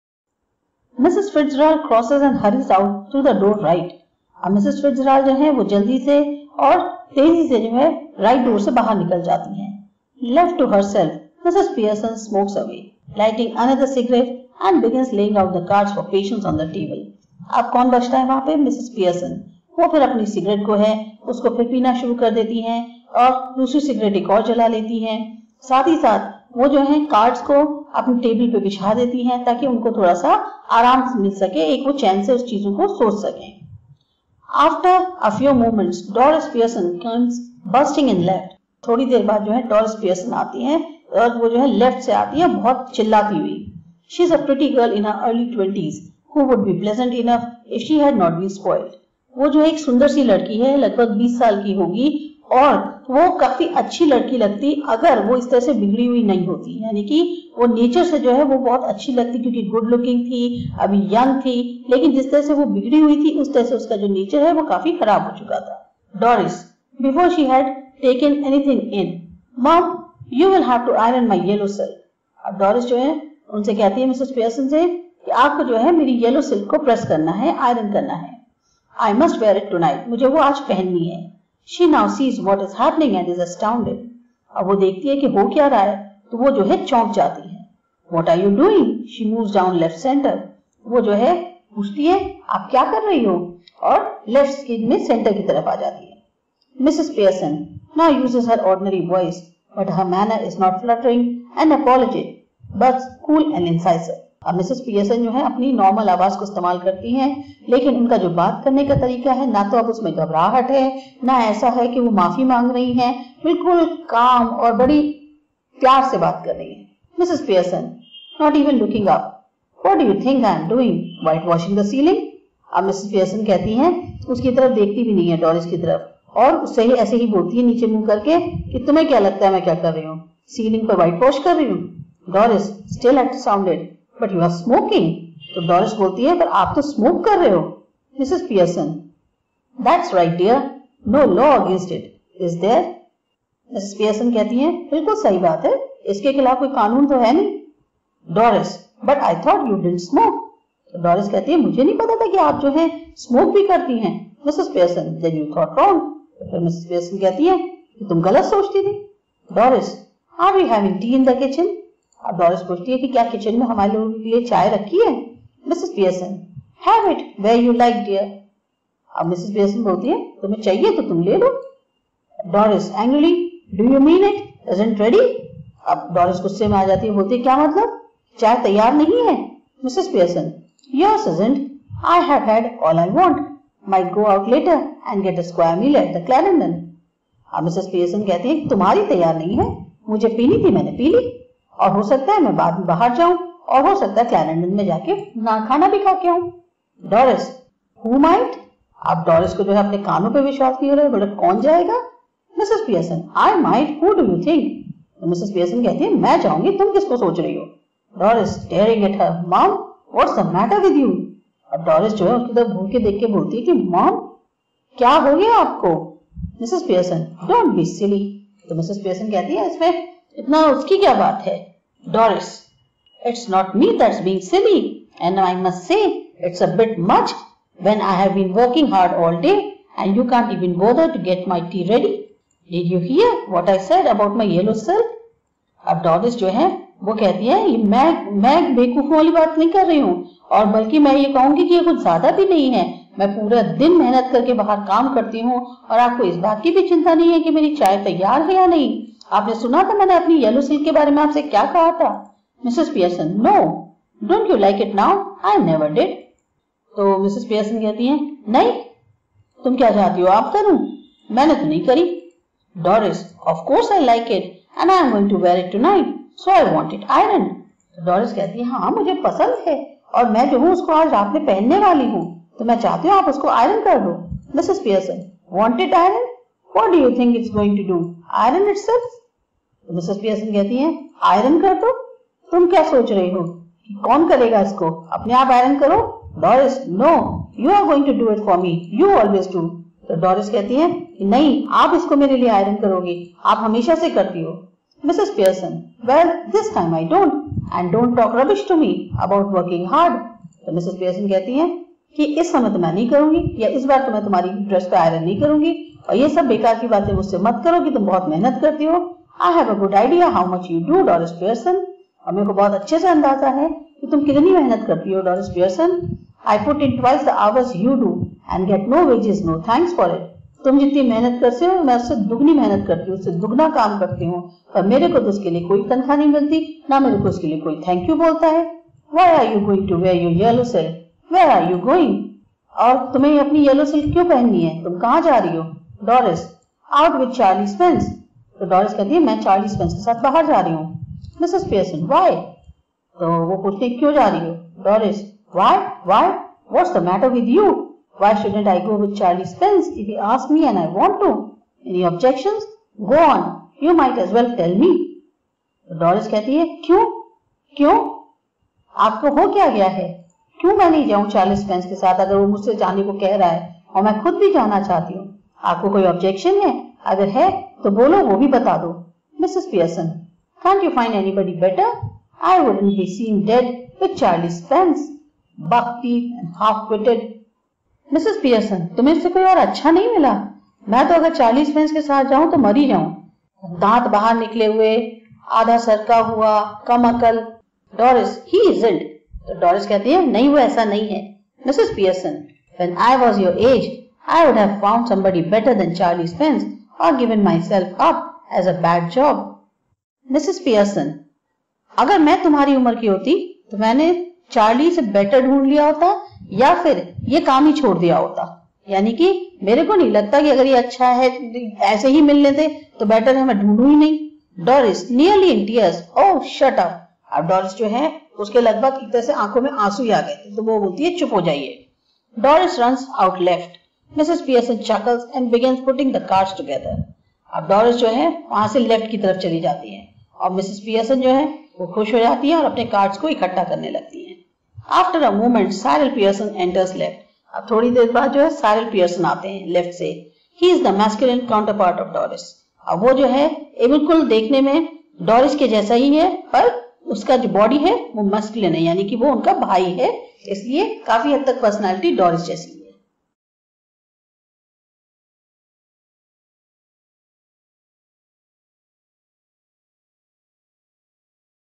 Ch 5.3 Mother's Day (English - Snapshots, CBSE, Grade 11) Easy Explanation in Hindi
Subject - English.
Book - Snapshots.
Chapter 5 - Mother's Day.

PHDMP1012-A03